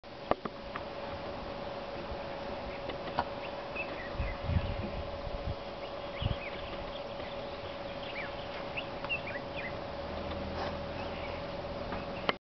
elephant walking
Elephant walking at Mahlasela waterhole in Tembe Elephant Park, South Africa.